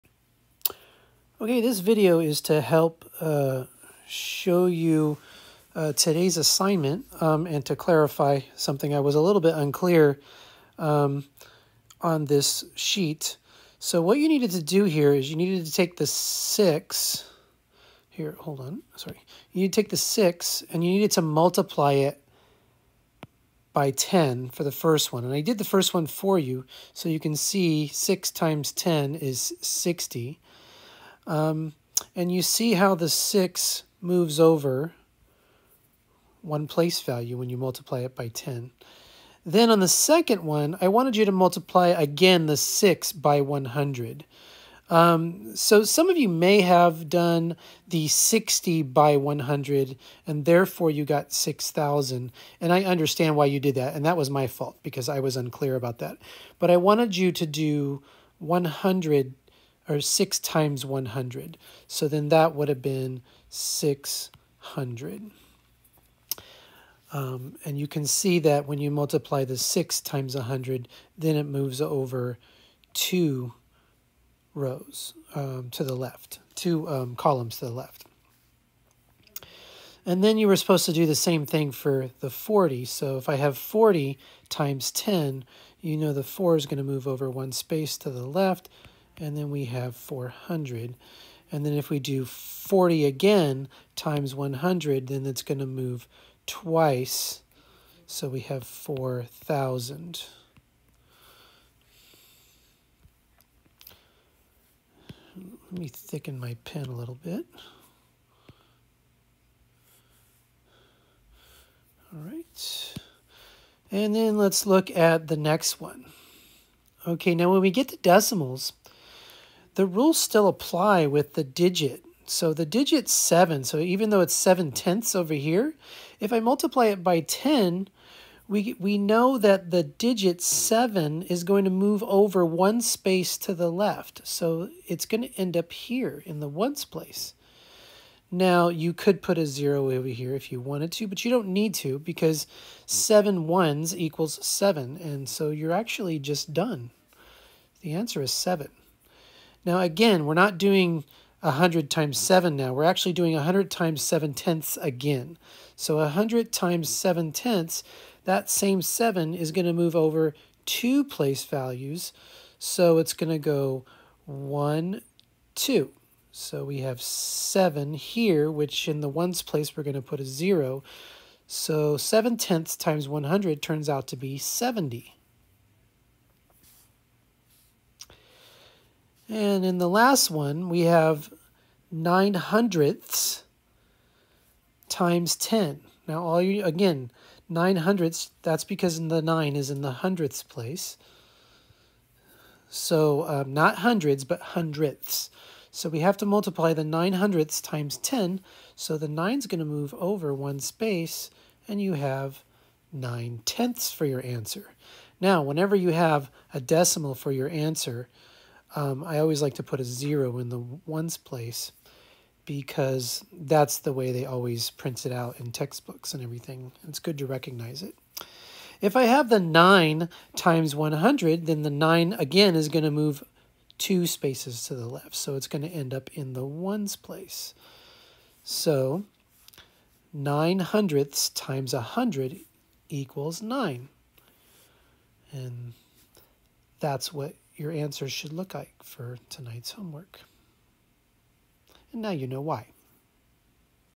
Multiplying by 10 and 100 Using the Place Value Chart
This lesson helps build a foundation for understanding base 10 number system.